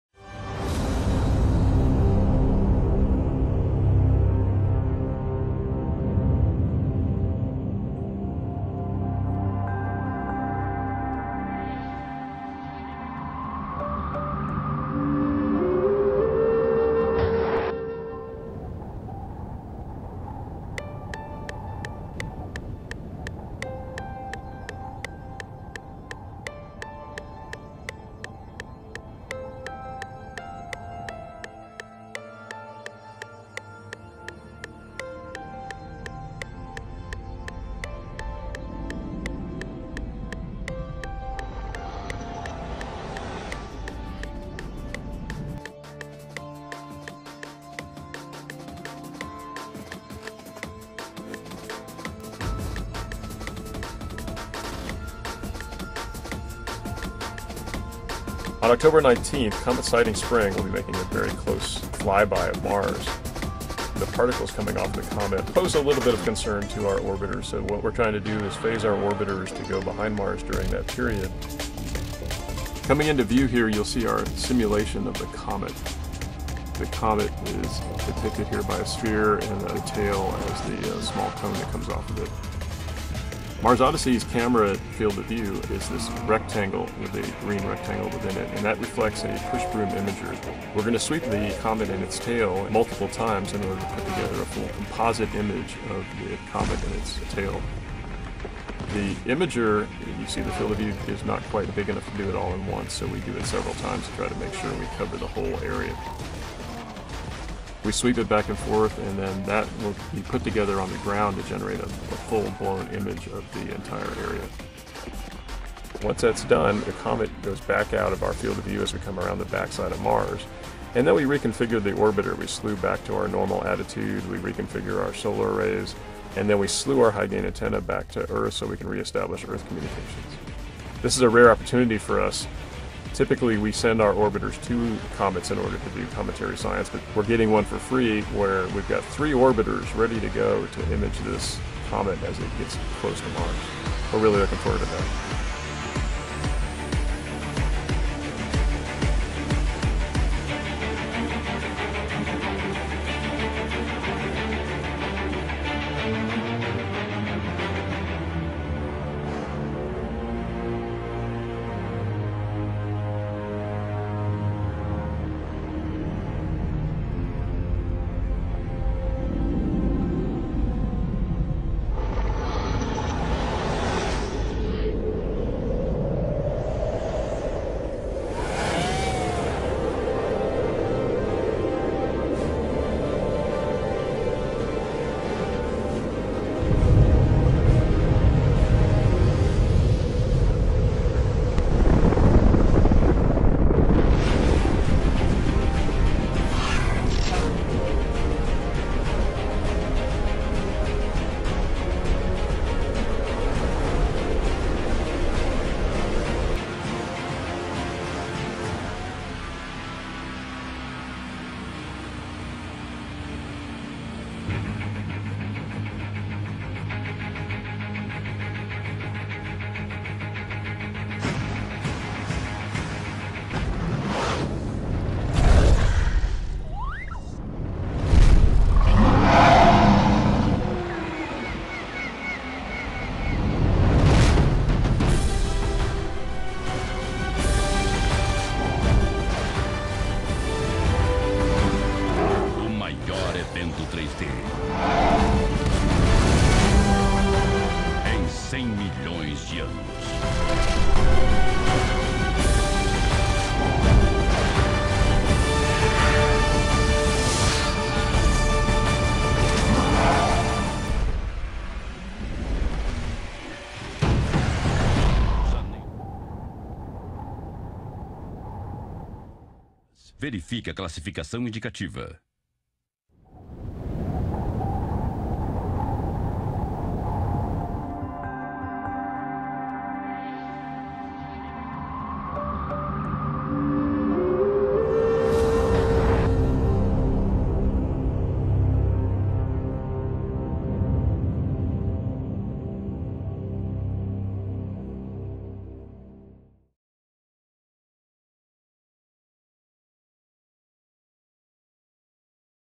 Fim 2015 - Impacto da cauda de Cometa em Marte pode lançar asteróides para Terra
Cometa gigante se aproxima de Marte após 1 milhão de anos.

Um cometa vai passar bem perto do planeta Marte neste domingo. Astrônomos e curiosos estão a postos para ver o fenômeno. Vale à pena conferir, porque o evento só deverá ocorrer de novo daqui a um milhão de anos.